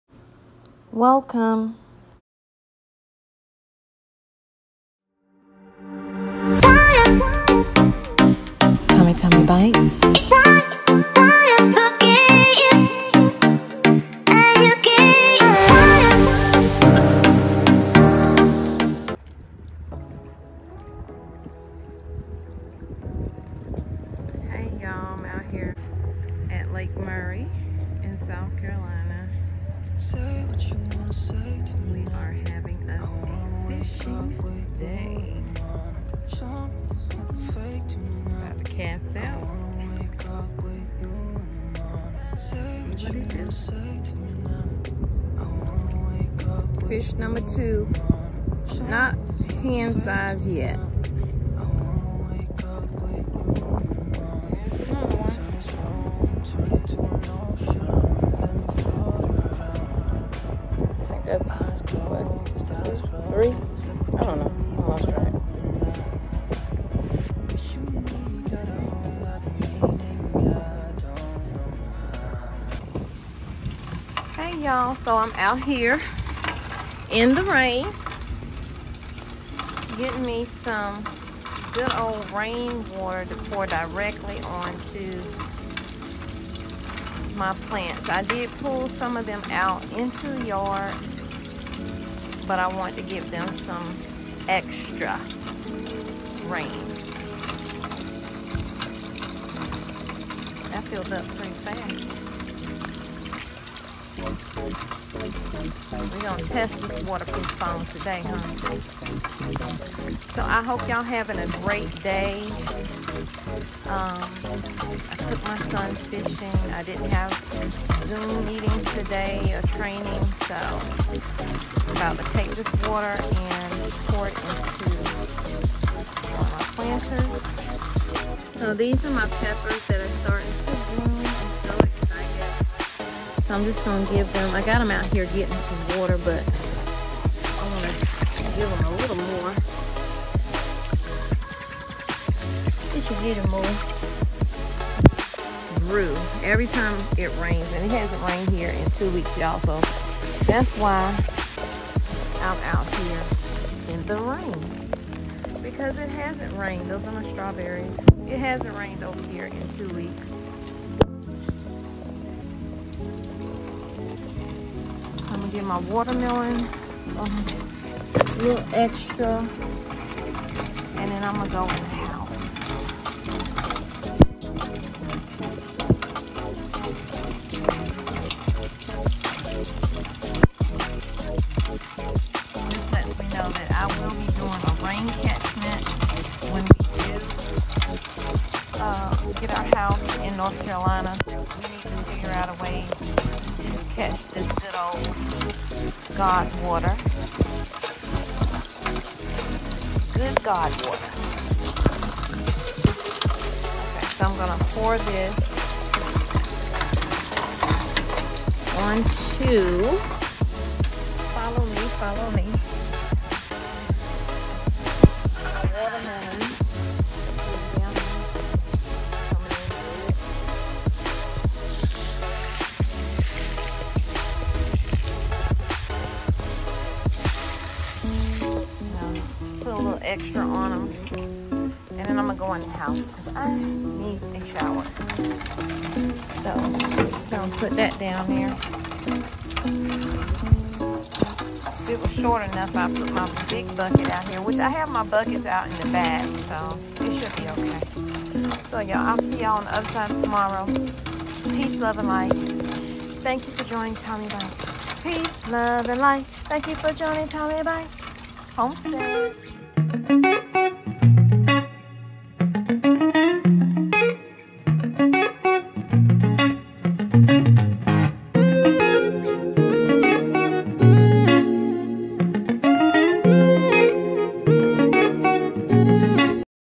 Rainwater Collection / Rainwater Harvesting/ Bluegill Fishing at Lake Murray Small Garden Tour
Welcome and Thank you💕 We Do Gardening, homesteading, cooking tutorials, eating mukbang.

⬇️ Today we are: Figuring out rain catchment system, Fishing For Bluegill at Lake Murray and small garden tour